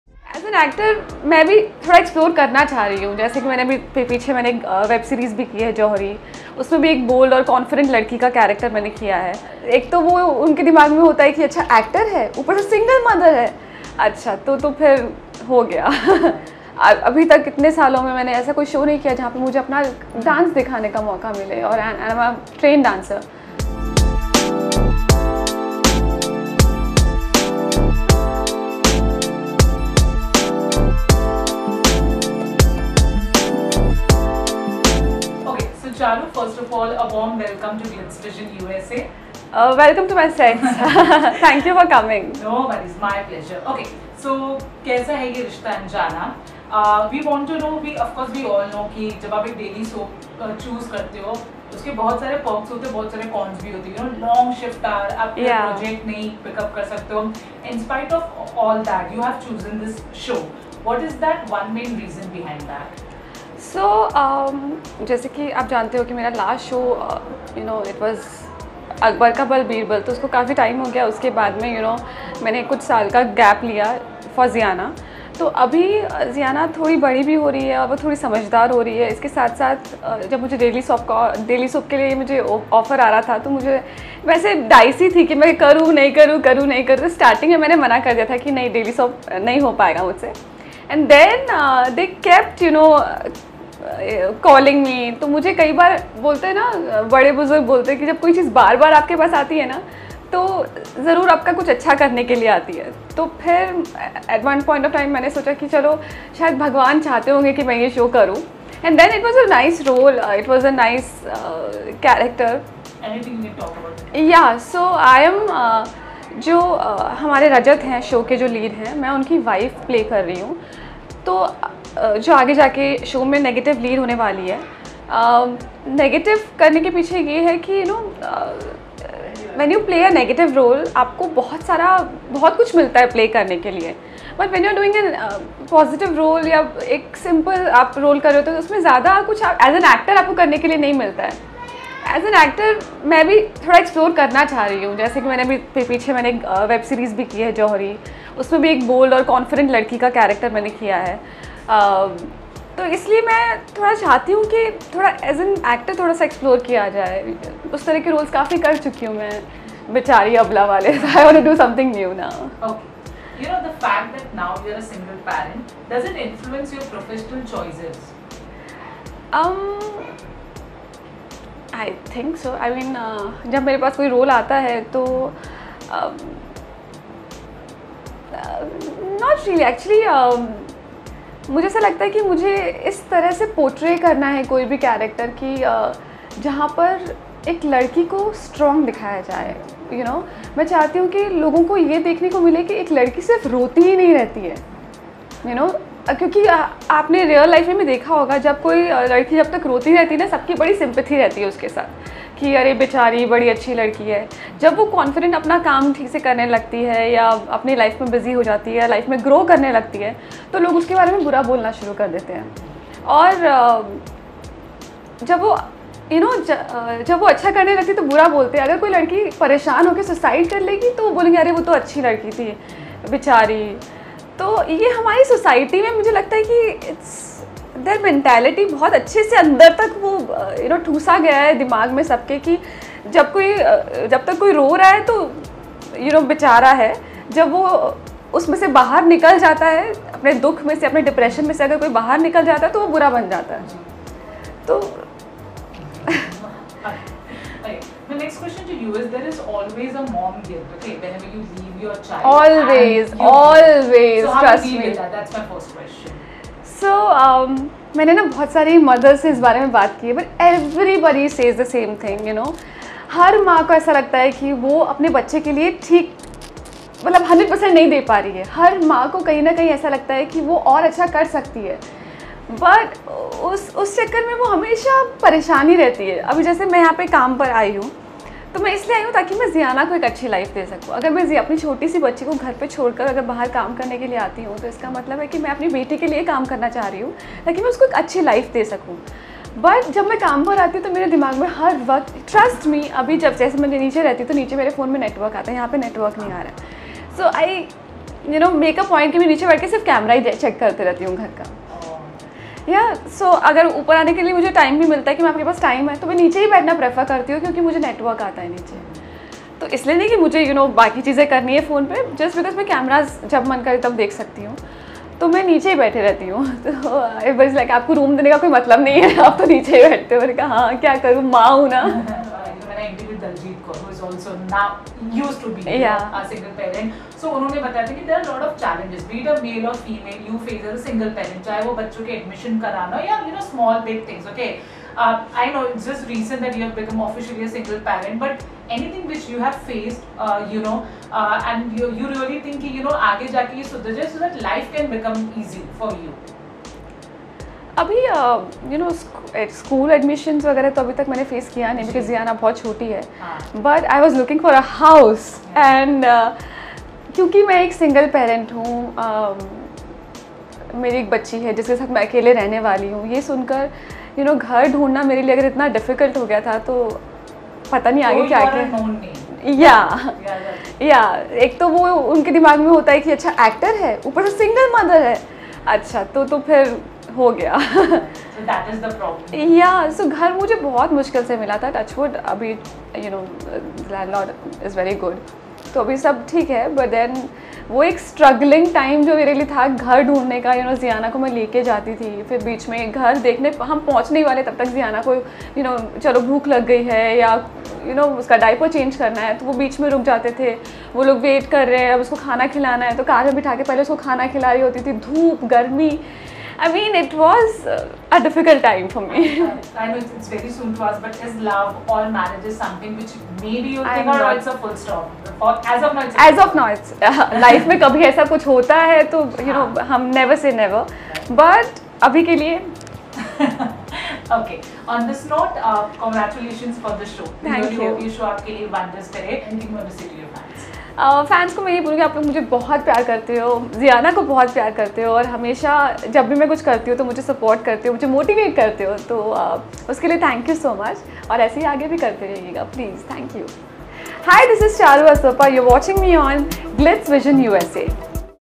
EXCLUSIVE! Charu Asopa ON Her New Show 'Kaisa Hai Yeh Rishta Anjana', Working As A Single Mom & More
Charu Asopa who is back on screen with Kaisa Hai Yeh Rishta Anjana gets candid about making her comeback, working as a single mom & more.

Interviewer: Kinnari Singh
Cameraman: Sairaj
Video Edited By: Mukul Kumar